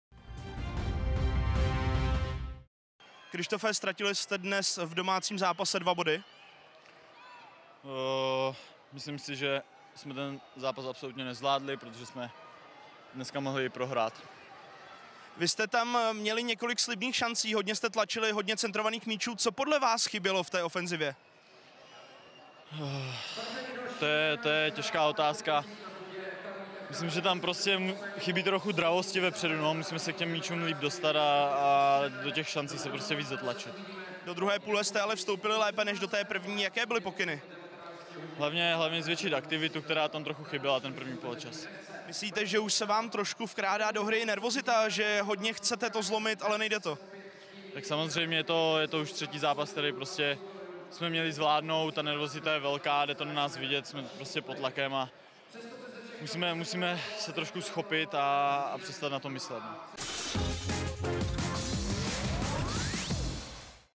Zklamaný Daněk: Absolutně jsme to nezvládli, musíme se vzchopit
Sparta doma jen remizovala se Zlínem 0:0 a potřetí v řadě ztratila body po nerozhodném výsledku. „Absolutně jsme ten zápas nezvládli, mohli jsme i prohrát. Je to už třetí zápas, který jsme měli zvládnout. Je to na nás vidět, nervozita je velká. Musíme se vzchopit, přestat na to myslet,“ hledal slova záložník Kryštof Daněk.